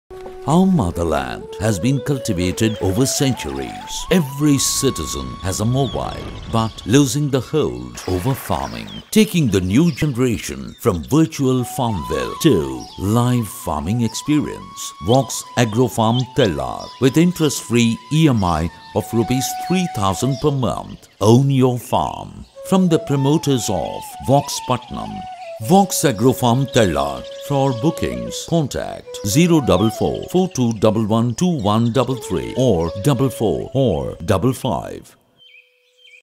Urban Farming Vox Agro Farm Thellar - English
Our Farm Features:
1. Drip Irrigation
2. Club house
3. Fishing Pond
4. Cattle Shed
5. Vermicompost Unit
6. Viewing Gallery
7. Poultry Farm
8. Ware House
9. 24/7 Security
10. Fully Fenced